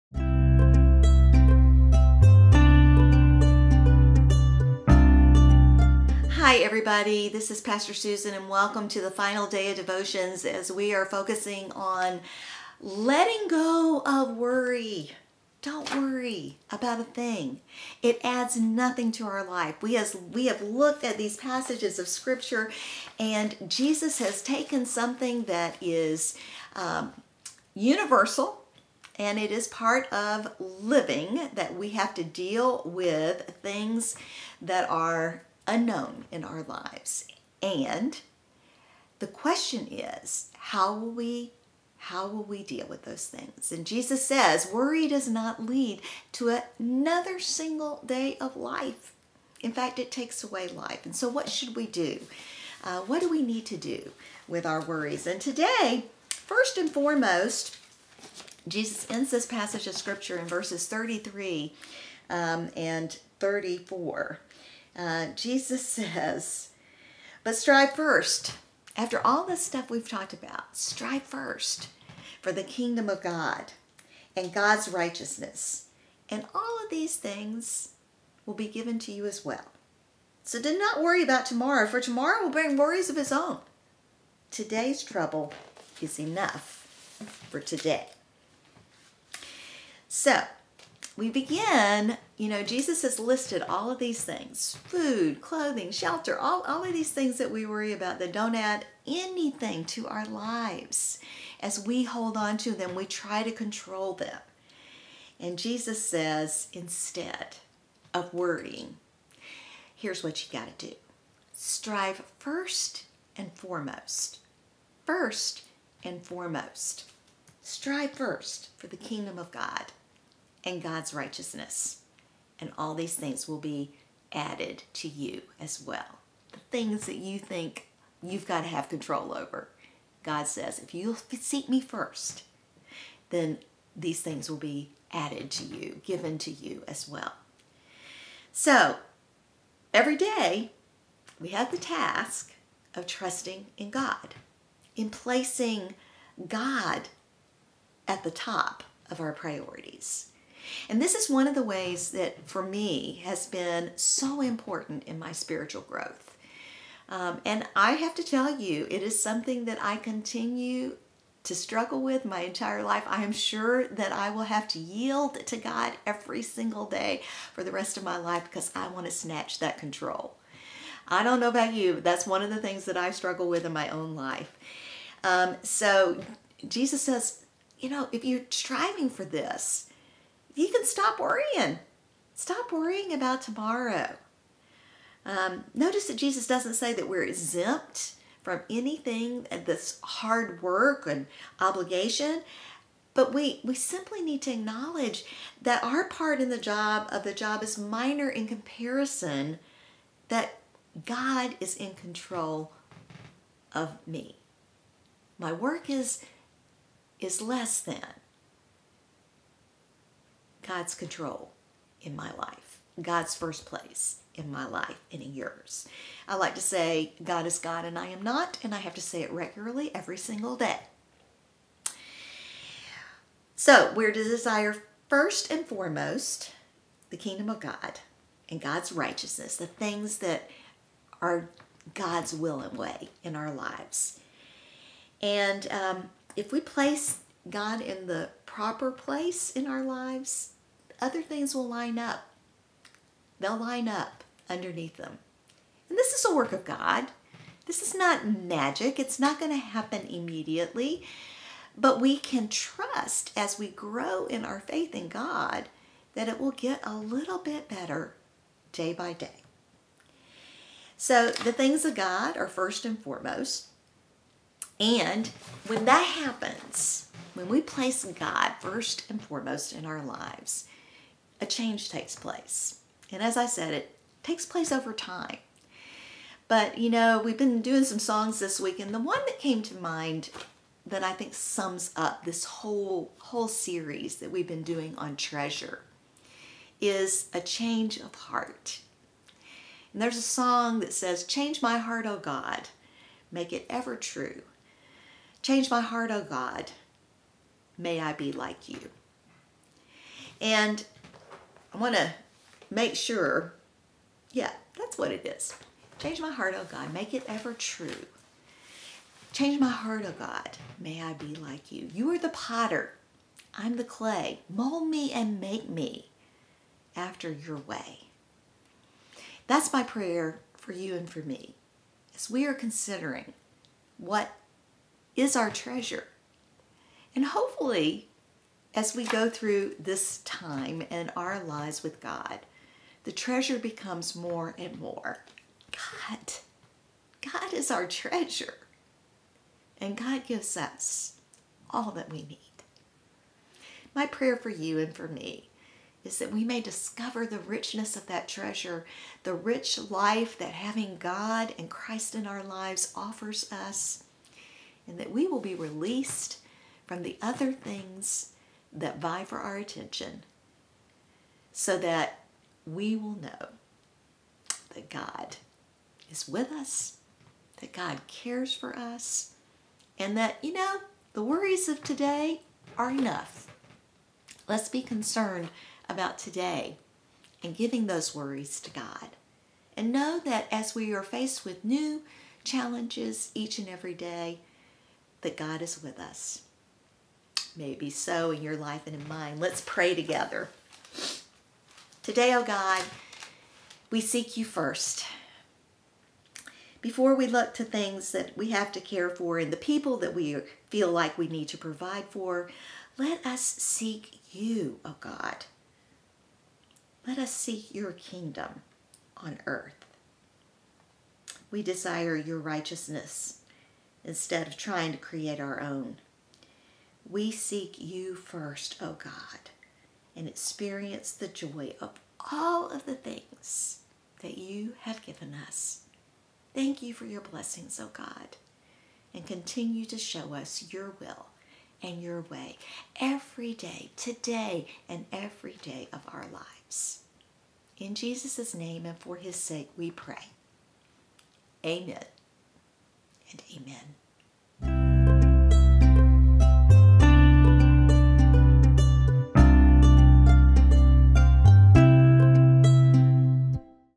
First and Foremost | Daily Devotion | Matthew 6:33-34
Sermon Series: Treasure
Theme: Don't Worry 'Bout a Thing